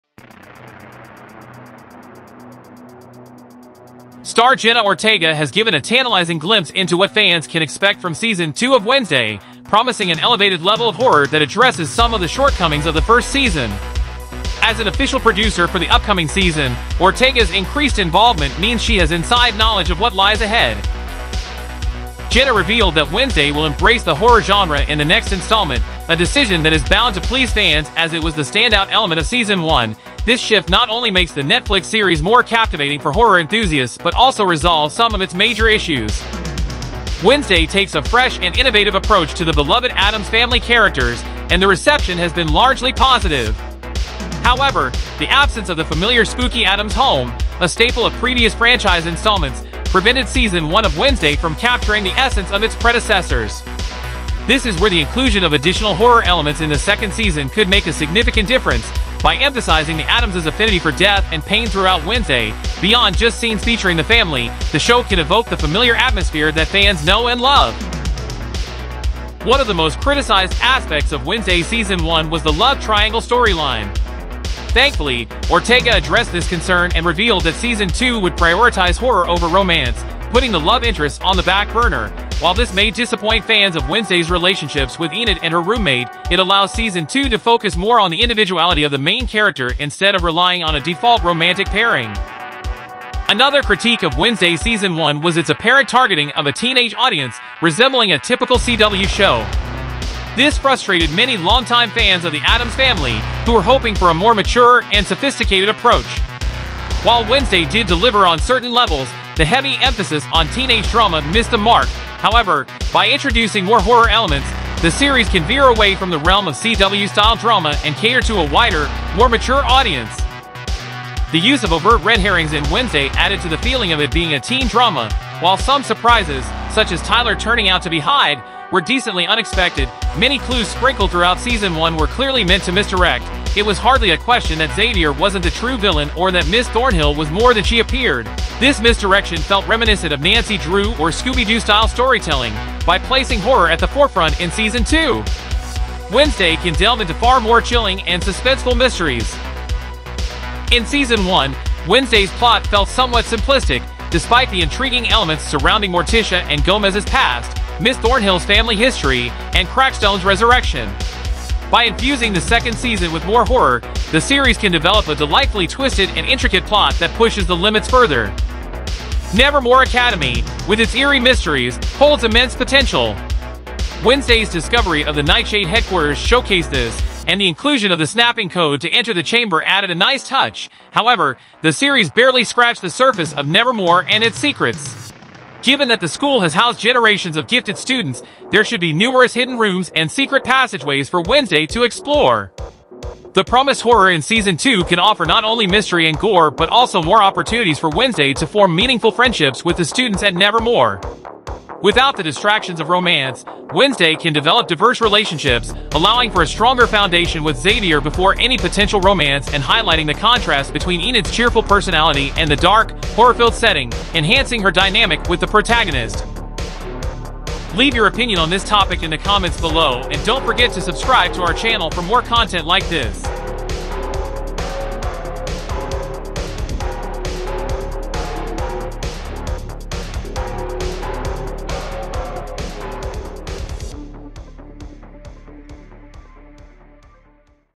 JENNA ORTEGA's Insights into WEDNESDAY Season 2
Welcome to VHS 64! In this video, we delve into the highly anticipated Wednesday season 2, where horror takes center stage and addresses the shortcomings of the first season. Join us as we discuss Jenna Ortega's insights as a producer, the thrilling twists and mysteries awaiting fans, and the potential for a darker and more sinister villain.